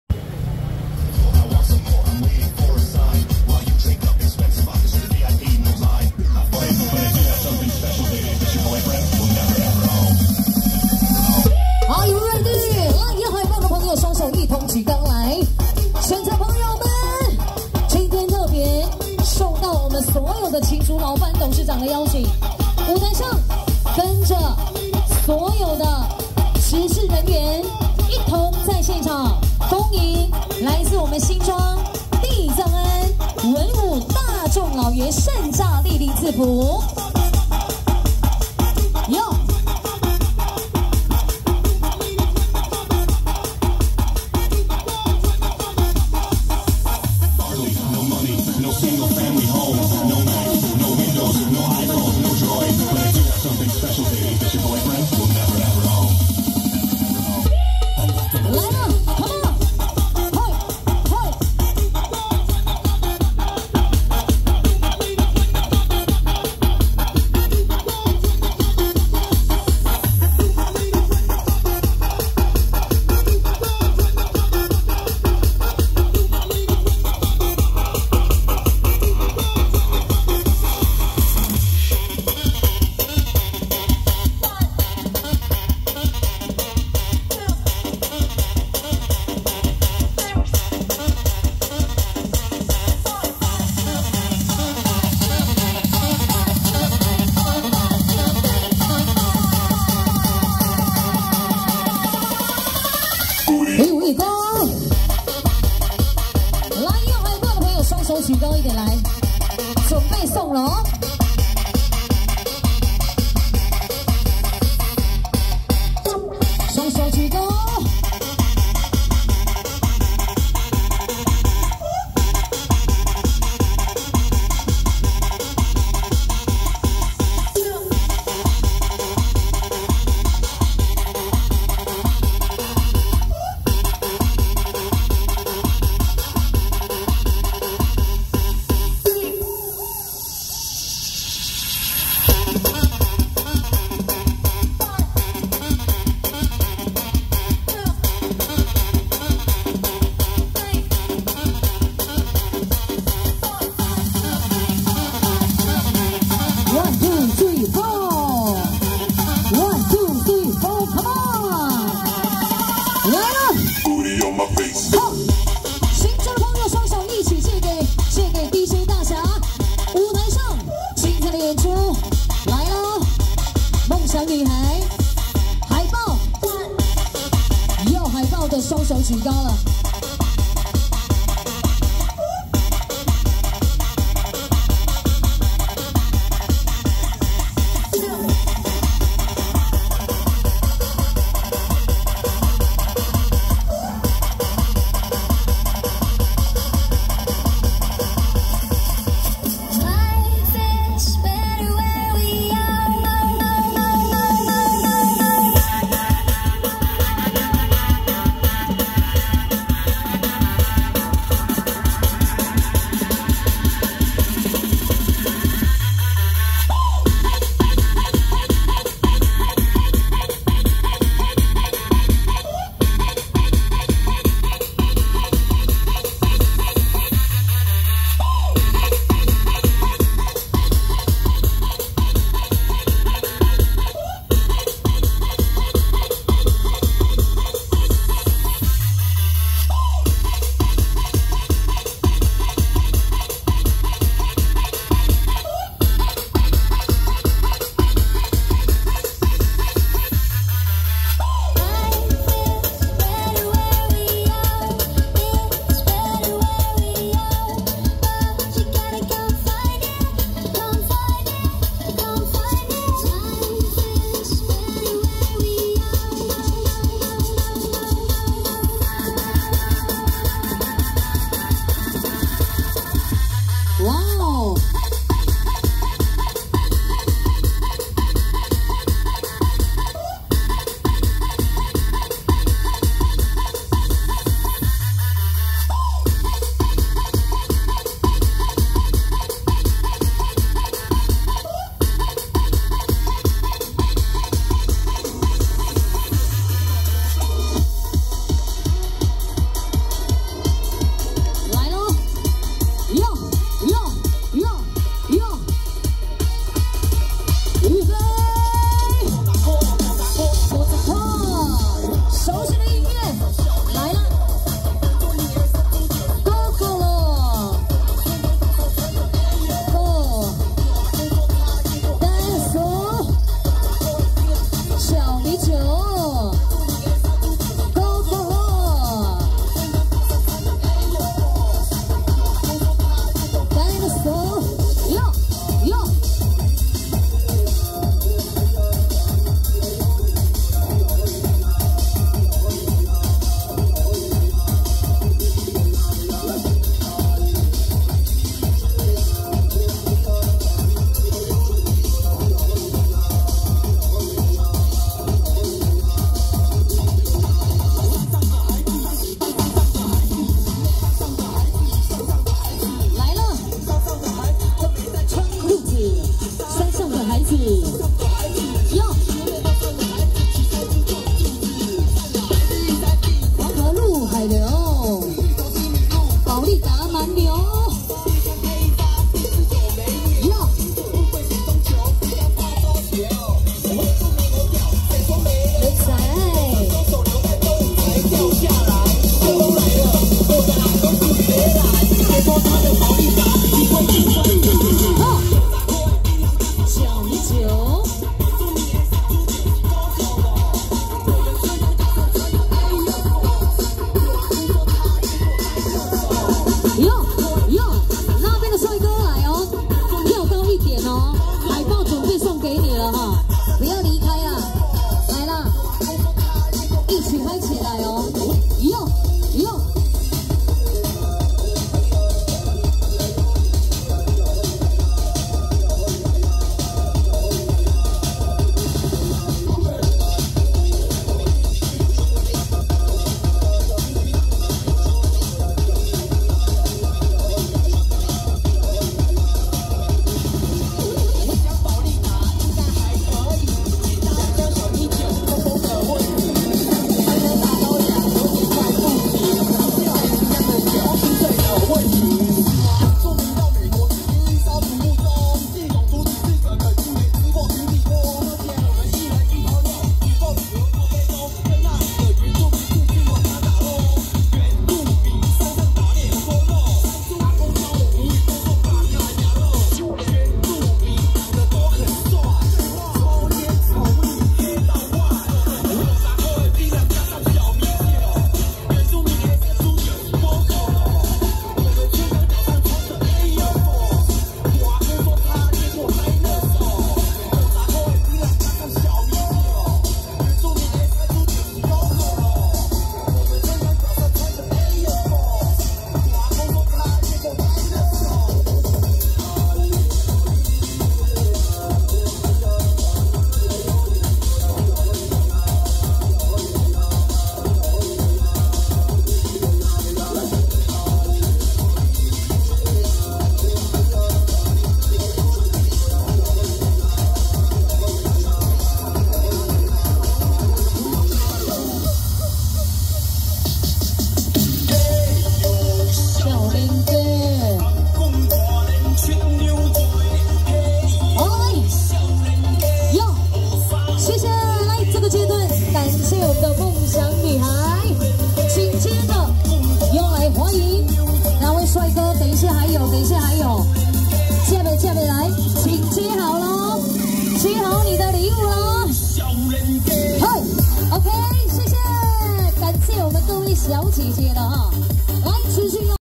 【夢想女孩舞團(夏.多.霏.粒.恩)(新莊聖易堂紅壇舞台)】~113新莊地藏庵恭祝文武大眾老爺聖誕暗訪遶境(頭前區)
夢想女孩舞團(夏.多.霏.粒.恩)(新莊聖易堂紅壇舞台)